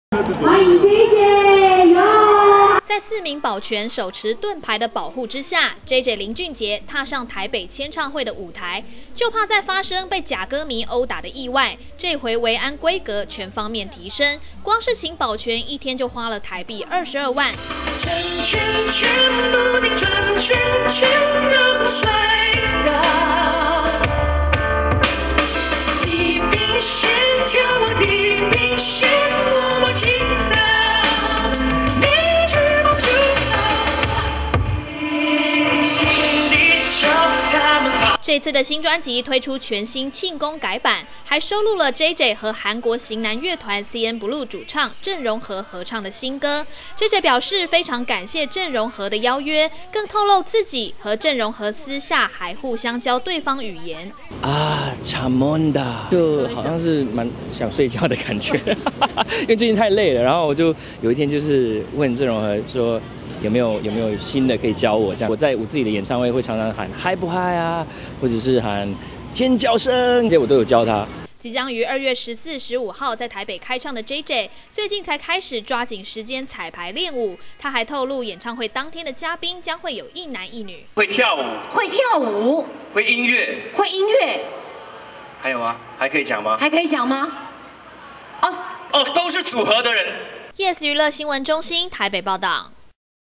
【5年前】JJ再簽唱四保鑣護身 教鄭容和喊尖叫聲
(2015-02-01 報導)

在四名保全手持盾牌的保護之下 JJ林俊傑踏上台北簽唱會的舞臺
就怕再發生被假歌迷毆打的意外 這回維安規格全方面提升
光是請保全一天就花了台幣22萬

這次的新專輯推出全新慶功改版 還收錄了JJ和南韓型男樂團
CNBLUE主唱鄭容和合唱的新歌 JJ表示非常感謝鄭容和的邀約
更透露自己和鄭容和私下還互相教對方語言
即將於2月14.15日在台北開唱的JJ
最近才開始抓緊時間彩排練舞
他還透露演唱會當天的嘉賓將會有一男一女